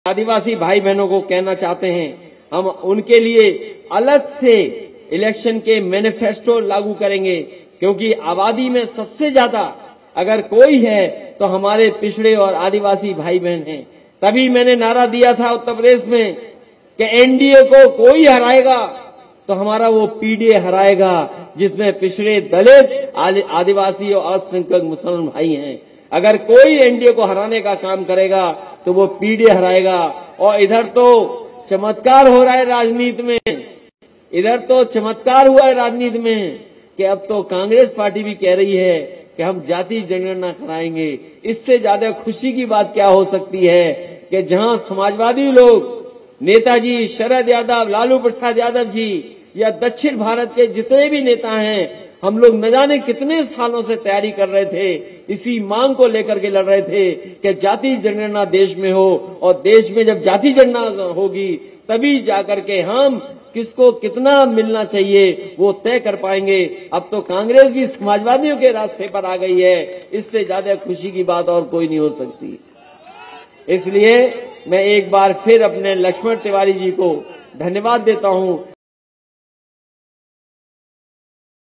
MP Election 2023: Akhilesh Yadav ने Caste Census पर Congress के बदले रुख पर क्या कहा? SP Rewa Rally
समाजवादी पार्टी के अध्यक्ष अखिलेश यादव ने रीवा में जनसभा को संबोधित करते हुए जातिगत जनगणना के मुद्दे पर कांग्रेस पार्टी के बदले रूख की जमकर तारीफ की। अखिलेश बोले- मुझे देखकर खुशी हो रही है कि कांग्रेस पार्टी भी अब समाजवादियों के रास्ते पर चलते हुए जातिगत जनगणना की बात कह रही है।